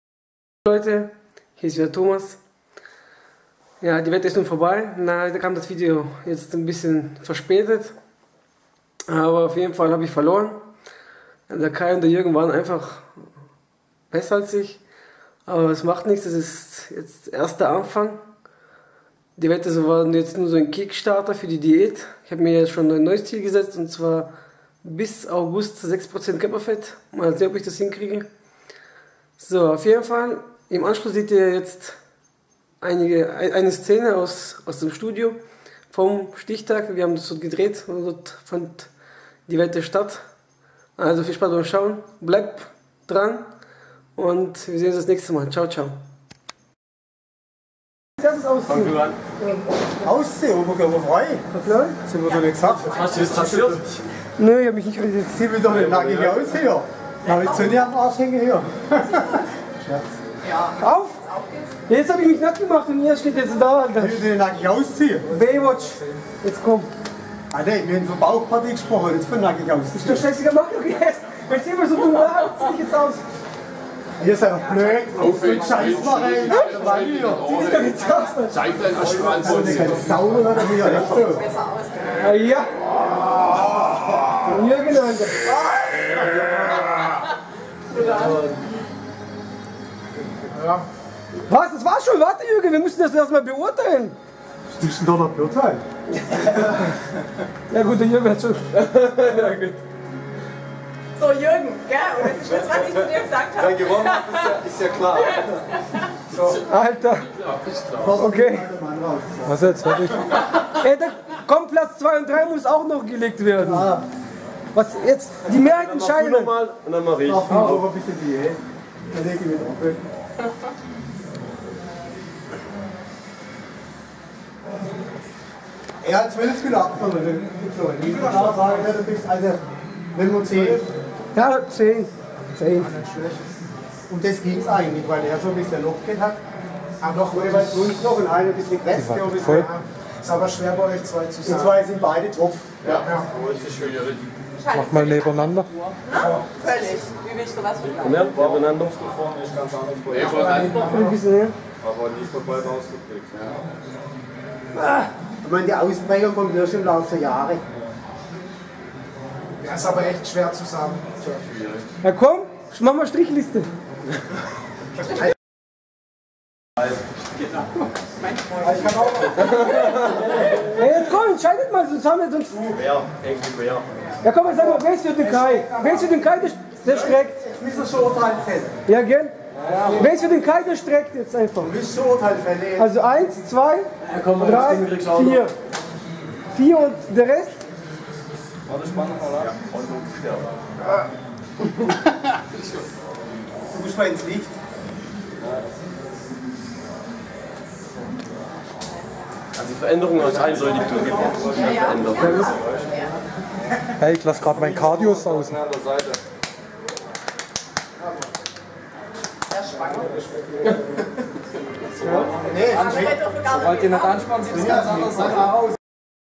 Thomas Kron - Die Entscheidung - Das Finale
Hallo Leute.
In dem Video seht ihr in welche Form wir es geschafft haben.
Leider habe ich verloren. Die Jungs waren einfach Top.
Egal! Das nächste Ziel steht bereits fest - bis August 6% Körperfett. Mal sehen ob ich das hinbekommen.
Also einfach weiter dran bleiben. Es werden noch einige Videos folgen.